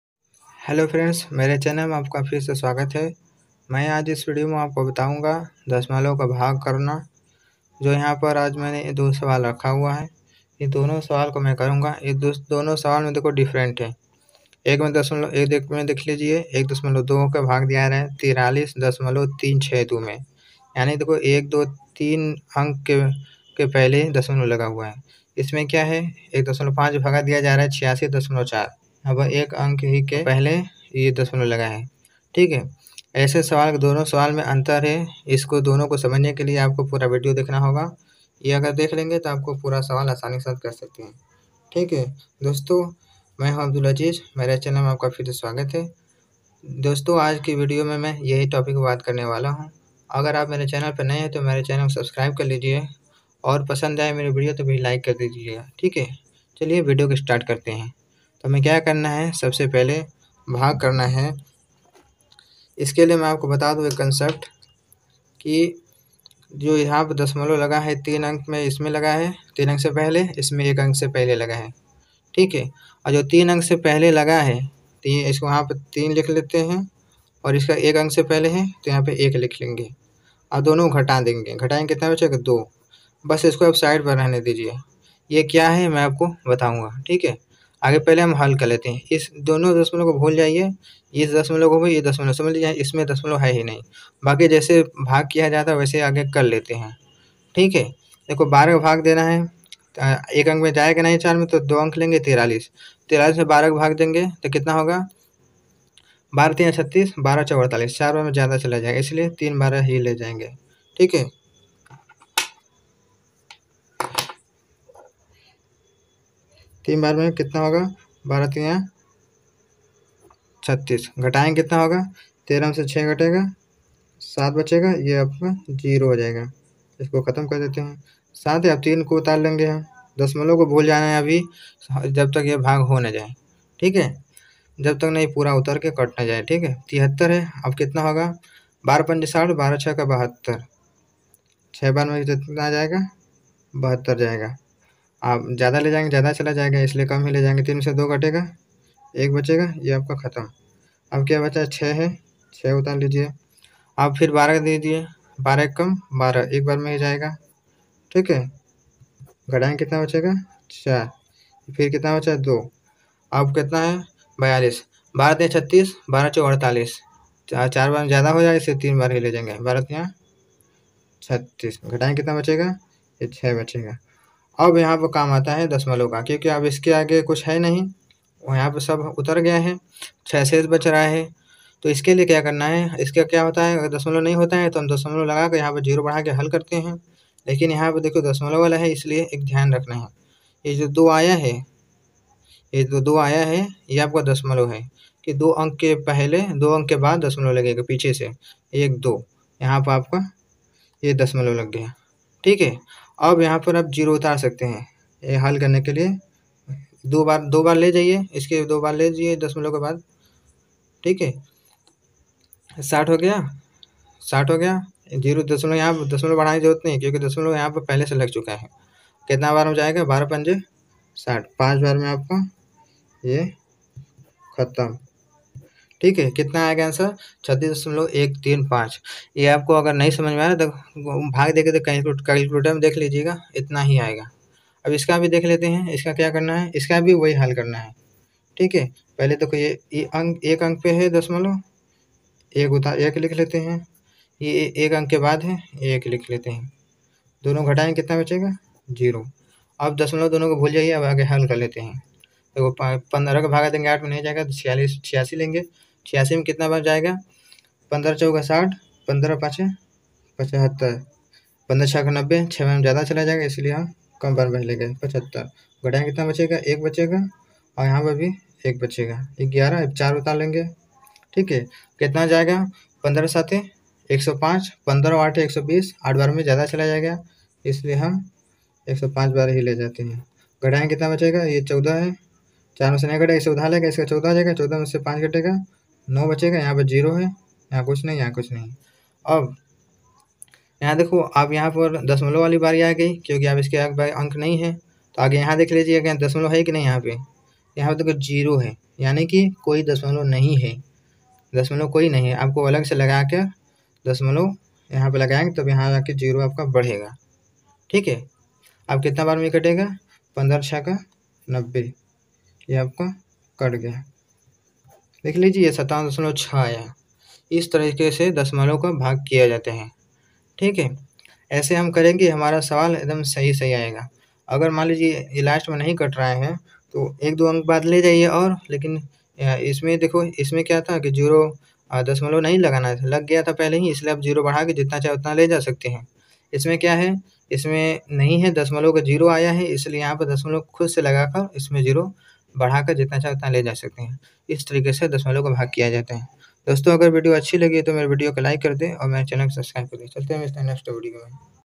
दशमलव संख्या का भाग करना सीखें | bhag kaise karte hain | division kaise karte hain | bhag kaise kare
video cover--

1. Bhag kaise banaen
2. Bhag kaise karte hain
3. Bhag banana sikhen